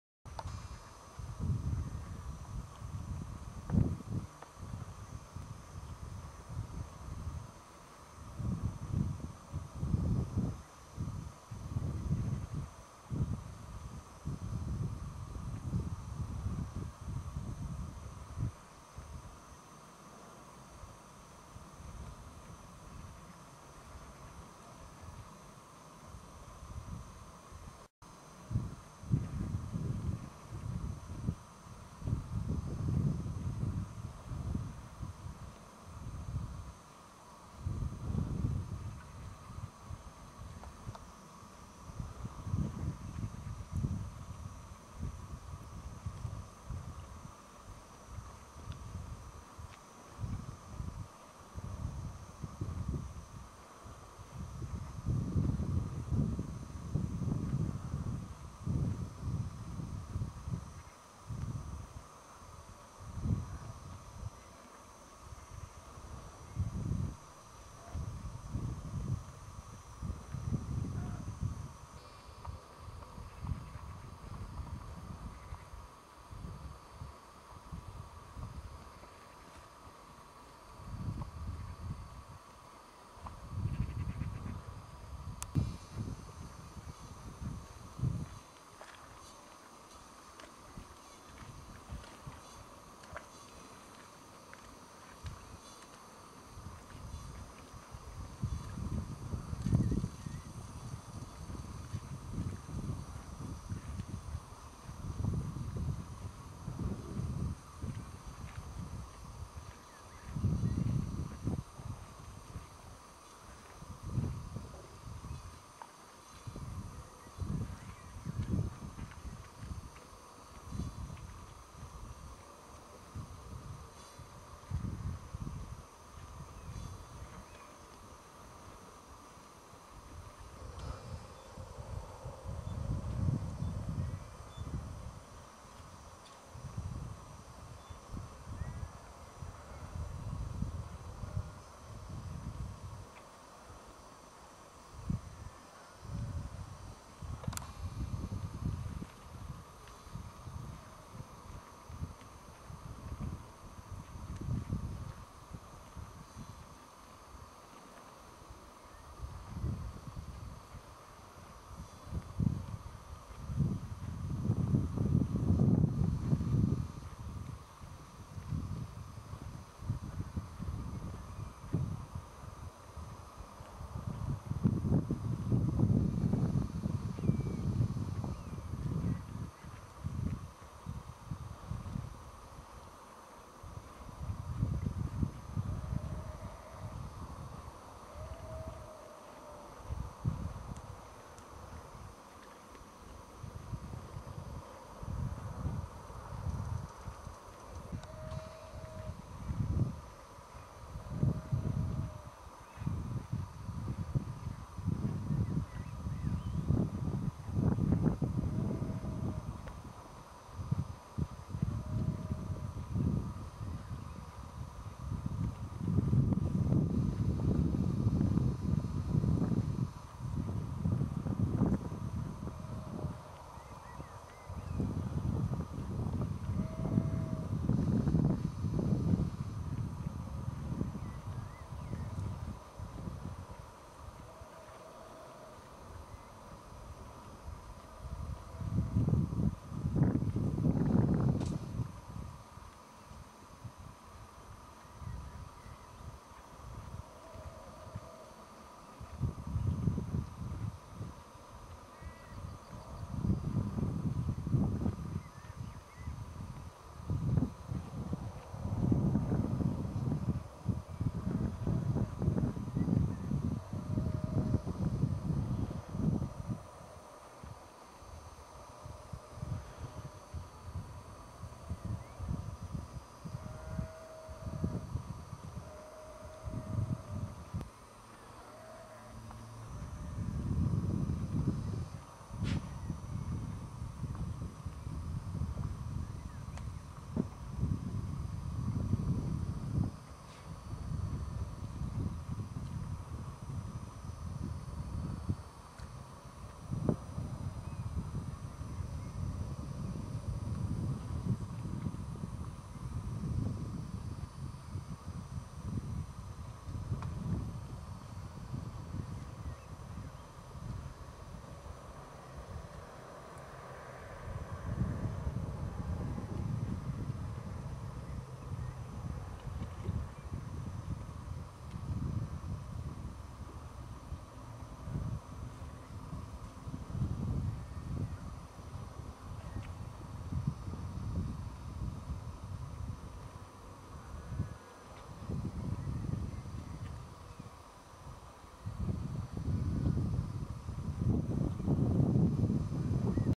Wild Scottish Badgers - trouble with cattle
The male badger is forced to give way to something even more black and white than him.  And just when he is being so 21st Century on the housework front.  Not sure why he tries to collect bedding in a thistle patch.